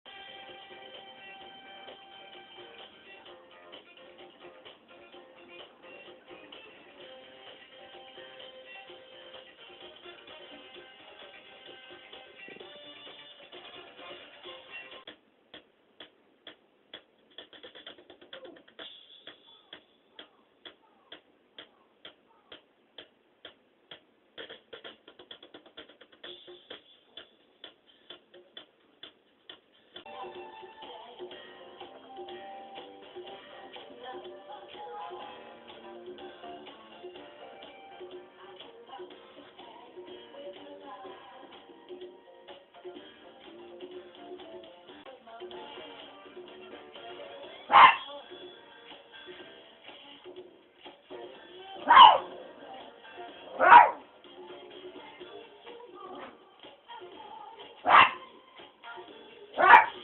2015 11/12 07：37~07：40 マルチーズのちずです。
遊んでます。最後に、元気よく吼えてます。